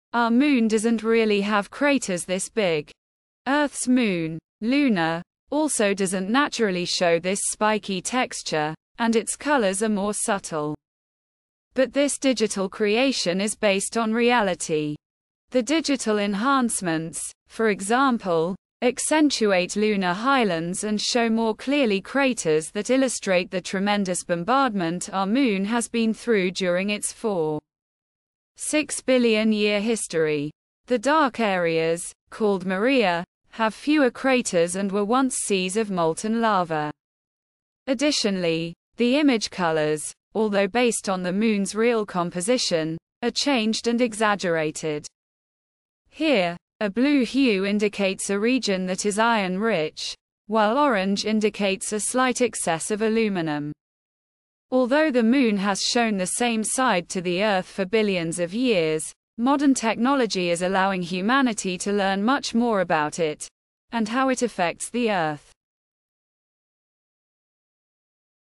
Exaggerated Moon
Our Moon doesn't really have craters this big.  Earth's Moon, Luna, also doesn't naturally show this spikey texture, and its colors are more subtle. But this digital creation is based on reality.  The featured image is a digital composite of a good Moon image and surface height data taken from NASA's Lunar Orbiter Laser Altimeter (LOLA) mission -- and then exaggerated for educational understanding.  The digital enhancements, for example, accentuate lunar highlands and show more clearly craters that illustrate the tremendous bombardment our Moon has been through during its 4.6-billion-year history.  The dark areas, called maria, have fewer craters and were once seas of molten lava. Additionally, the image colors, although based on the moon's real composition, are changed and exaggerated.  Here, a blue hue indicates a region that is iron rich, while orange indicates a slight excess of aluminum.  Although the Moon has shown the same side to the Earth for billions of years, modern technology is allowing humanity to learn much more about it -- and how it affects the Earth.

Credit & Copyright: N/A